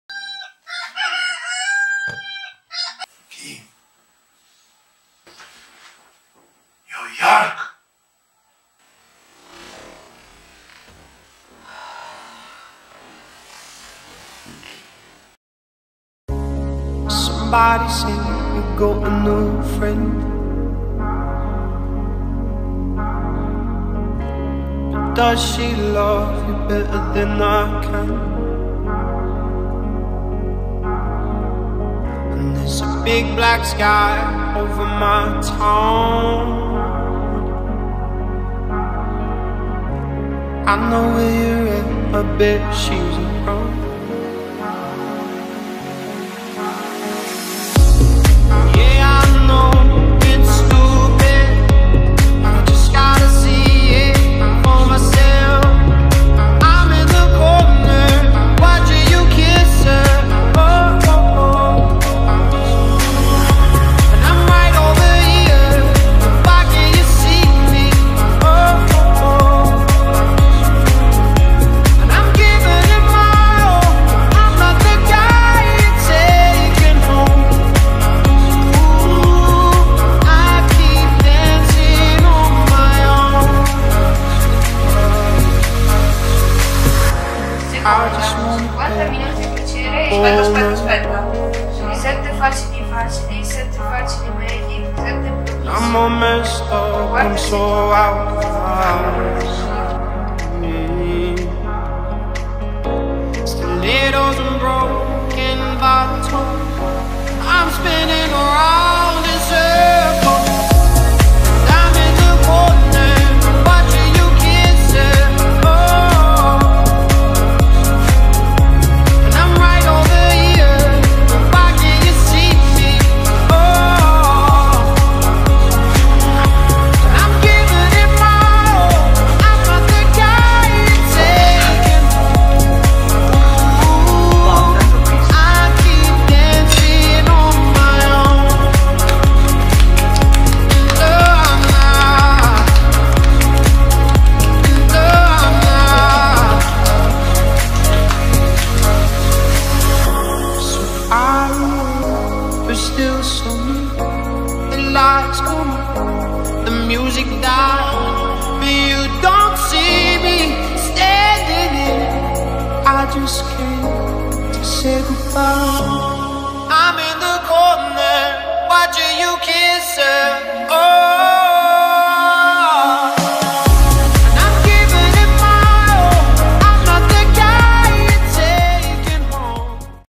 statenisland
For those who don't know, I'm partecipating in a contest called MyFututreSelfNY: the 3 winners will run the New York City Marathon, each runner's dream. Here's a short clip about my first 3 weeks of training, called "Staten Island". Follow me in this wonderful journey, and, most of all, run happy! :-))